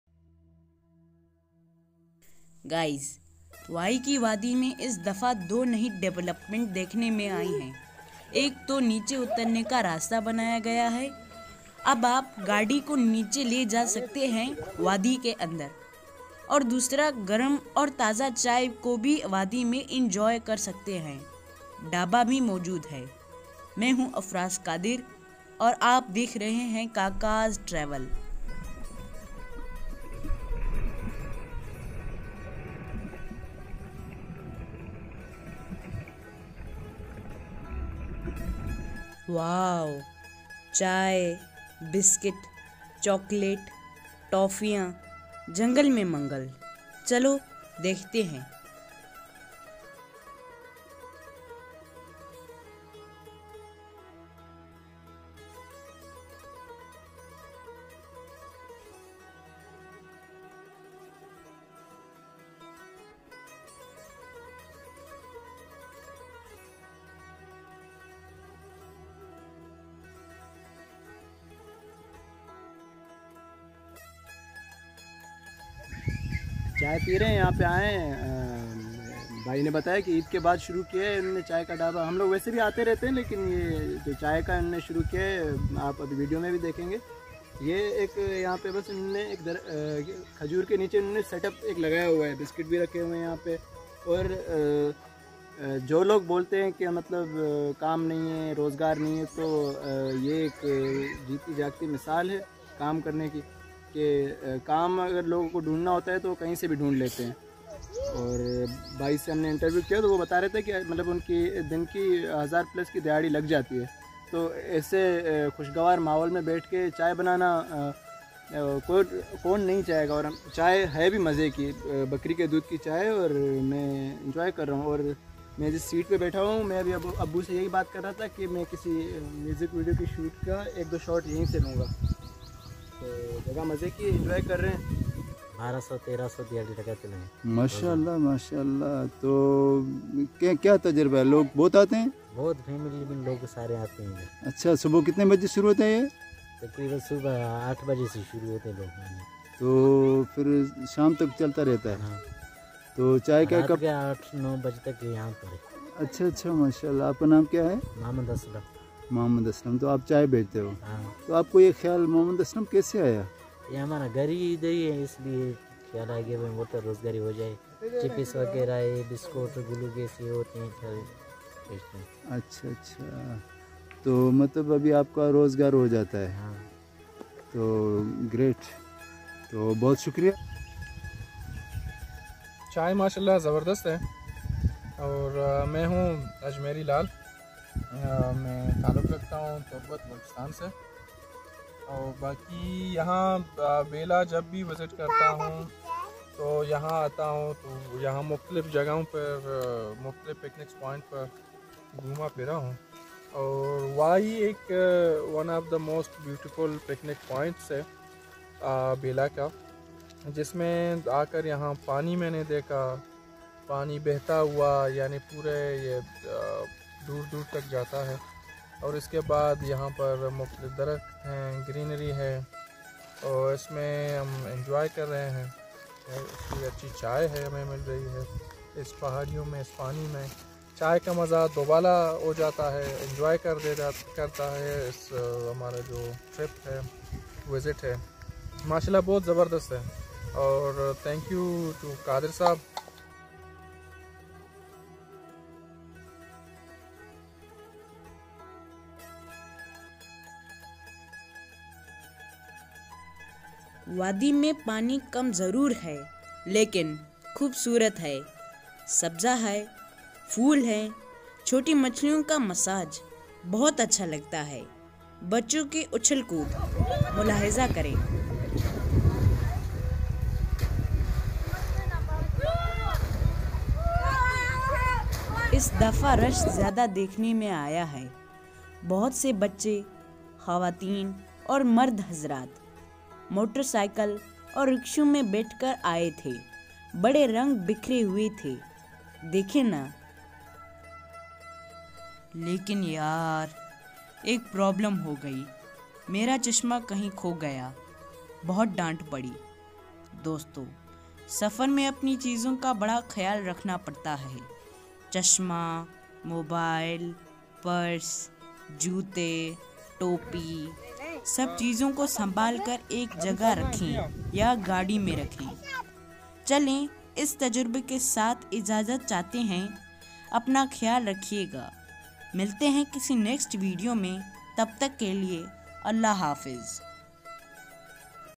The Beautiful Valley of Bela Balochistan | Wahi
Kaka and his sons, along with their family and friends, travel as Kakas Travel. Travel to discover the hidden Balochistan and Pakistan. Strive to discover new things through travel and bring up social issues.